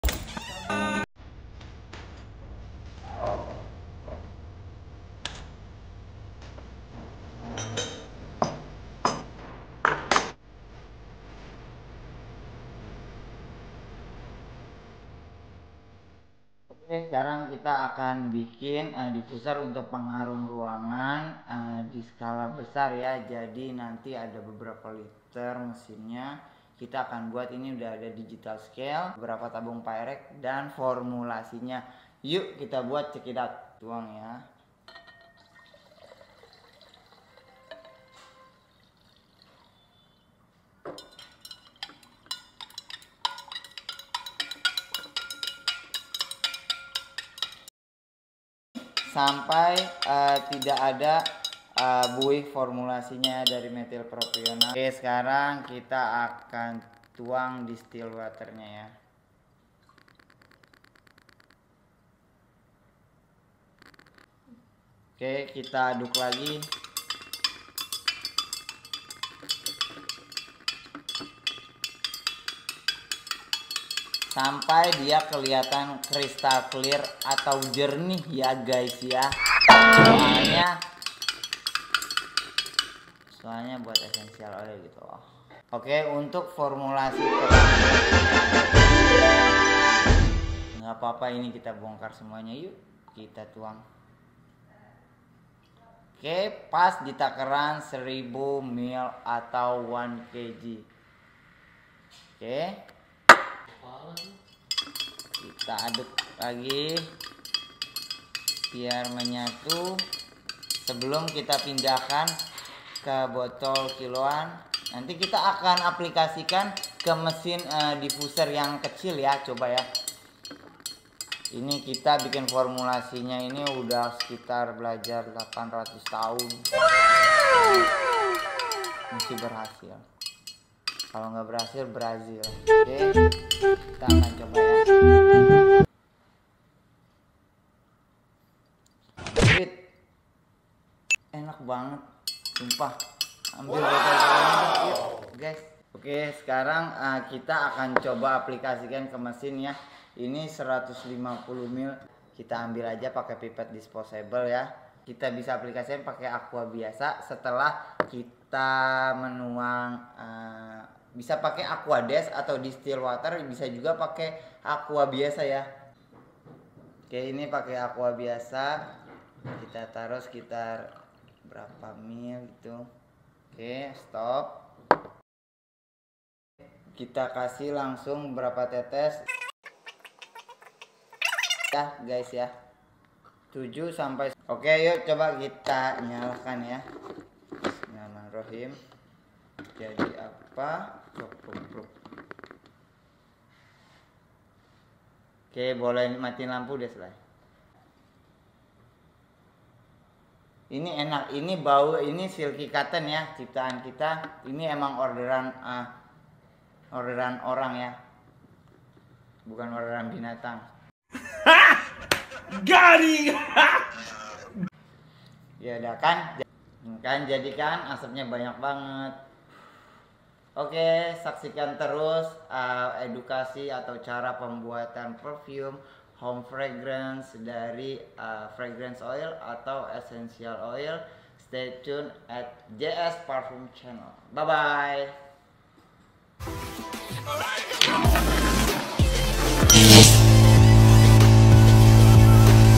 Cara Membuat Diffuser / Air Humidifier menggunakan essential oil atau Fragrance Oil
Pembuatan air humiddfier pewangi ruangan menggunakan essential oil atau fragrance oil

By
Jakarta Scent Parfum